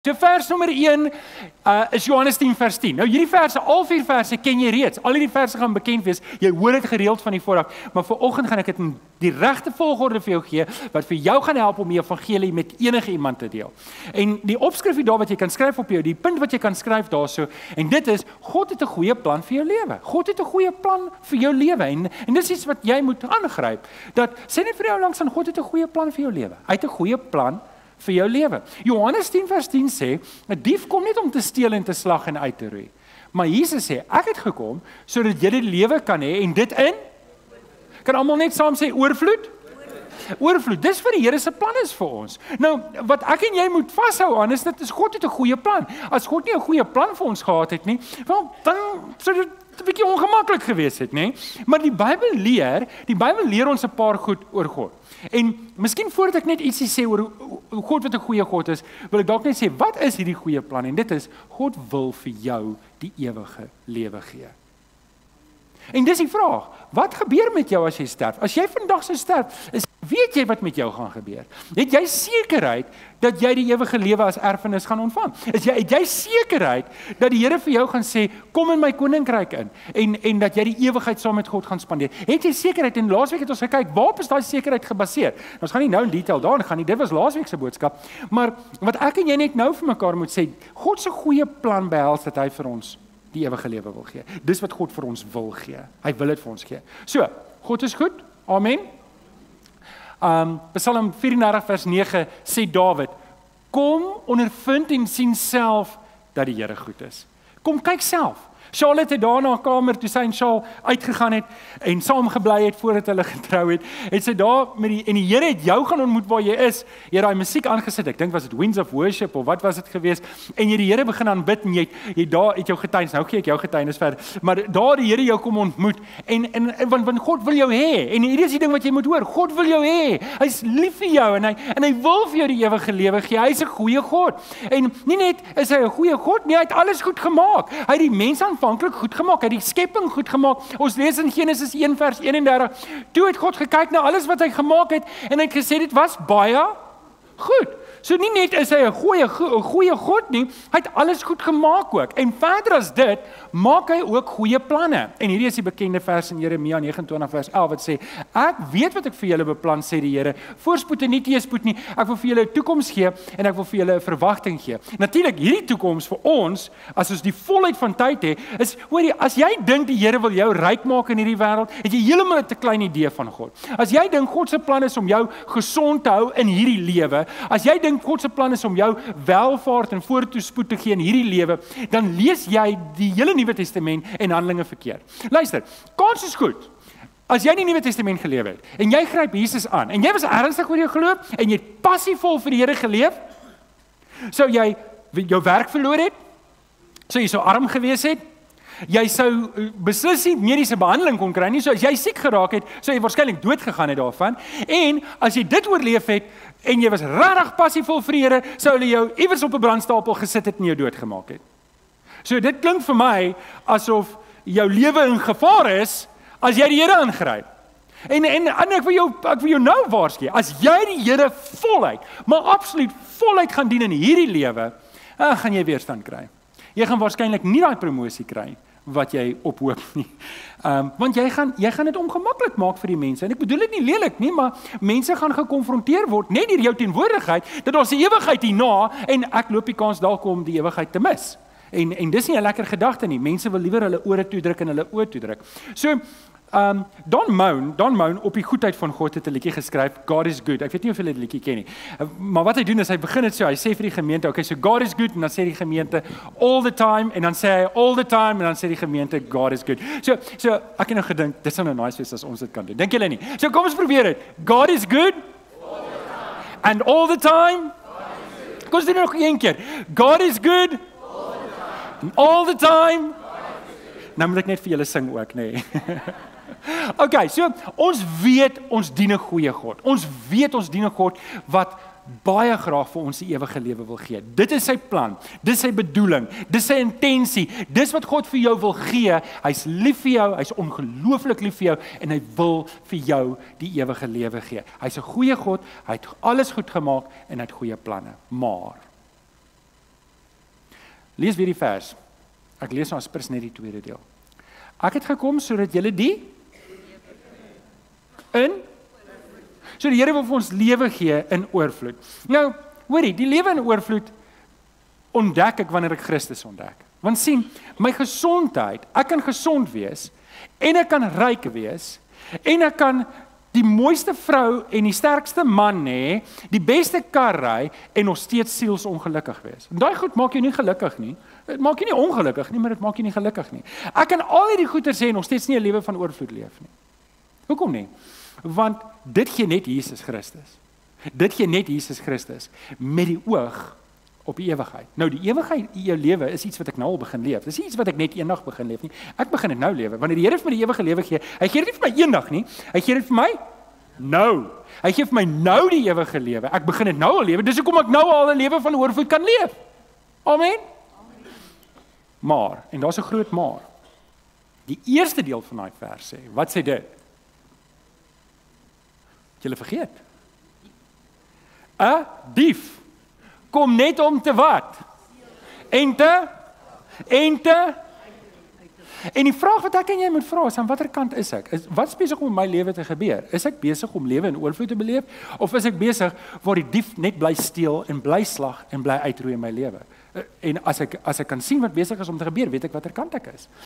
Eenvoudig Evangelie: God het `n goeie plan vir jou lewe
Hierdie is 'n uitknipsel van die preek: Die Evangelie in 4 Verse - Deel die evangelie eenvoudig en maklik | BUURTWAG. (Vers 1)